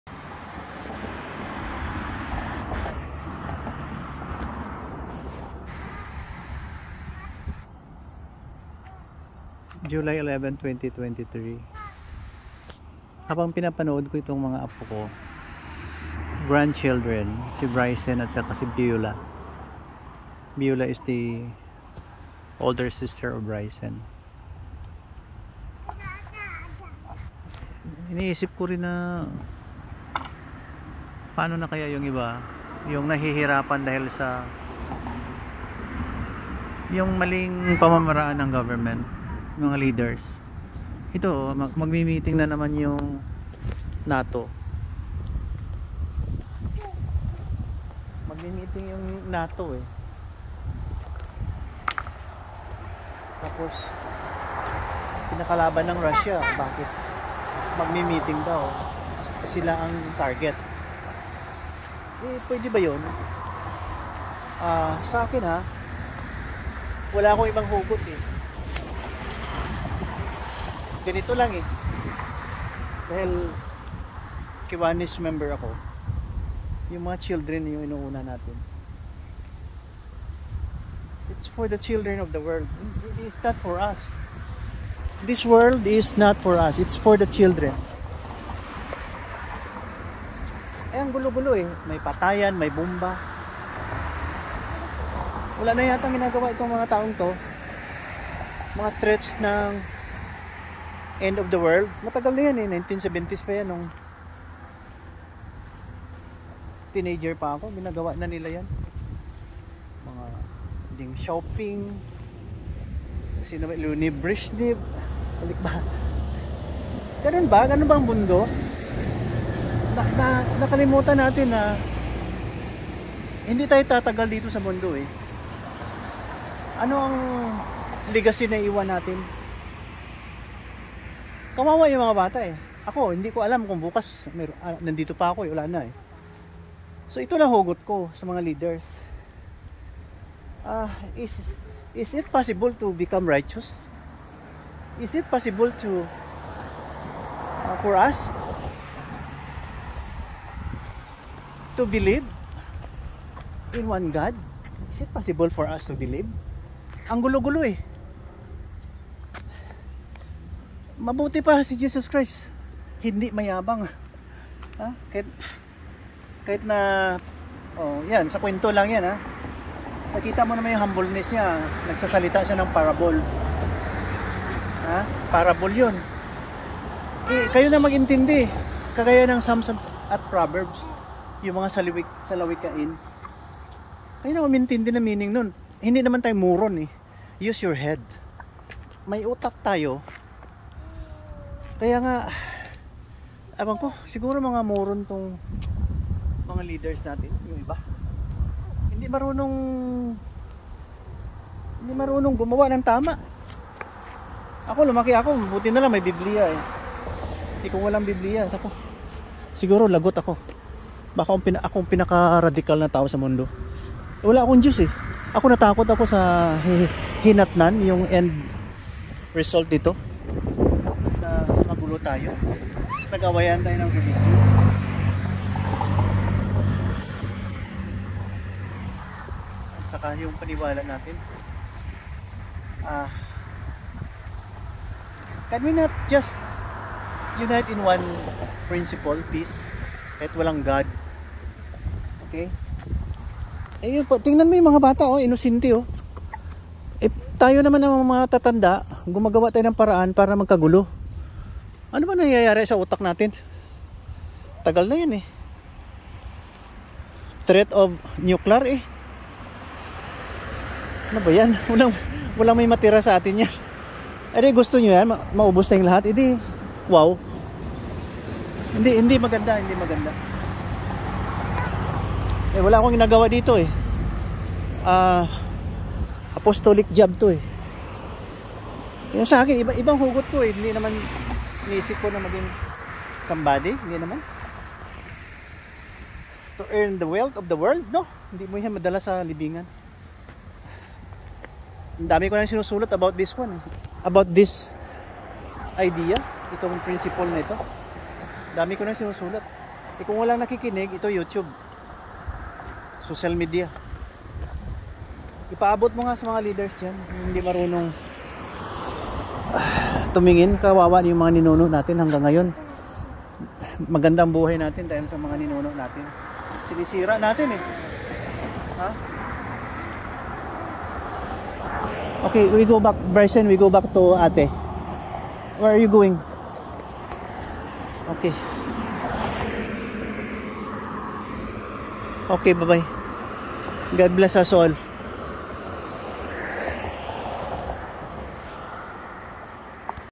Breaking!Filipino Kiwanian Member& Grandpa👴Against the War.Protect the Children of the World🌎71123
Breaking!
Filipino Kiwanian Member and
Grandpa👴is

Against the War.

instead proiritizes & Protect the Children of the World🌎

uploaded raw video.l & un-edited@London City Ontario Province Canada 🇨🇦  🍁

71123